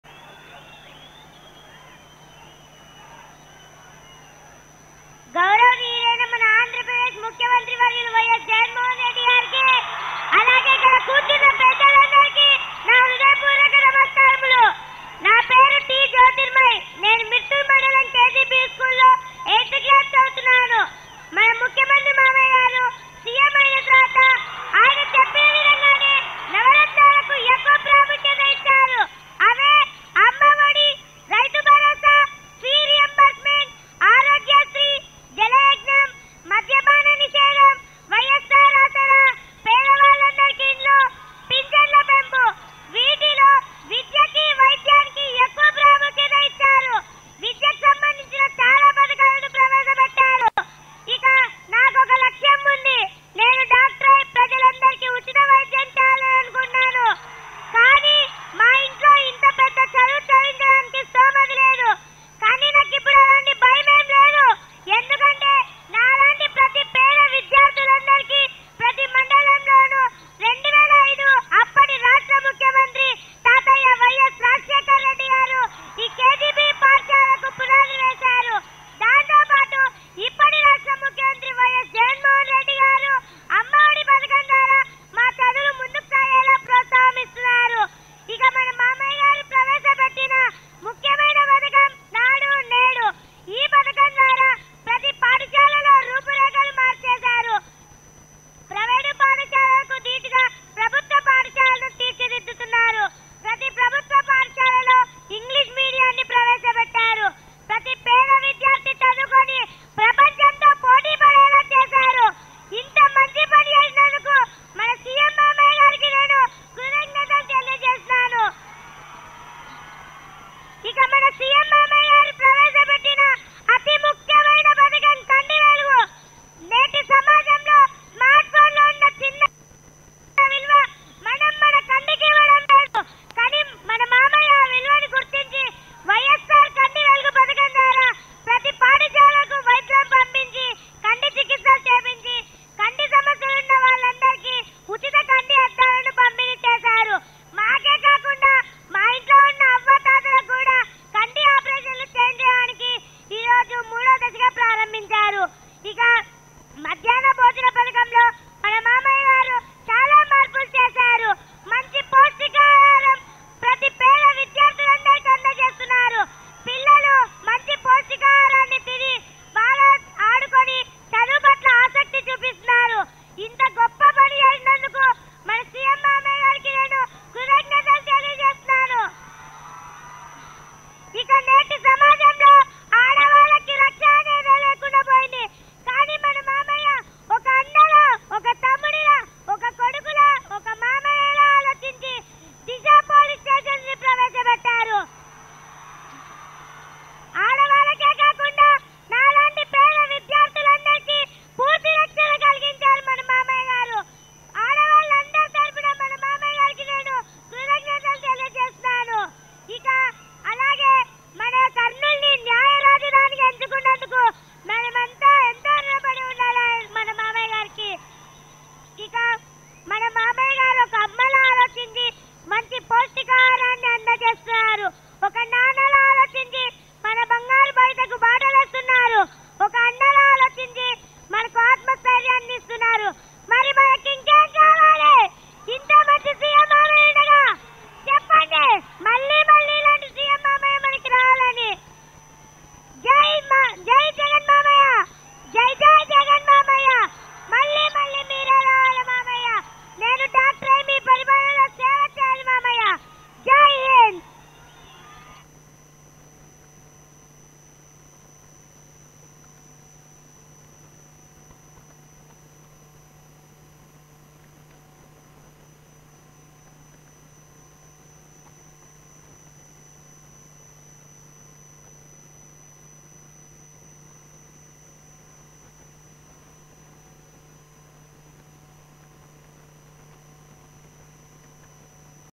Jagan | Appreciated | Kurnool student Jyothirmayi | Speech Adurs | ఈ చిన్నారి స్పీచ్ కి జగన్ ఫిదా
చిన్నారి జ్యోతిర్మయి స్పీచ్ కి.. కంటతడి పెట్టిన జగన్.. ఖచ్చితంగా అందరూ వినాల్సిన స్పీచ్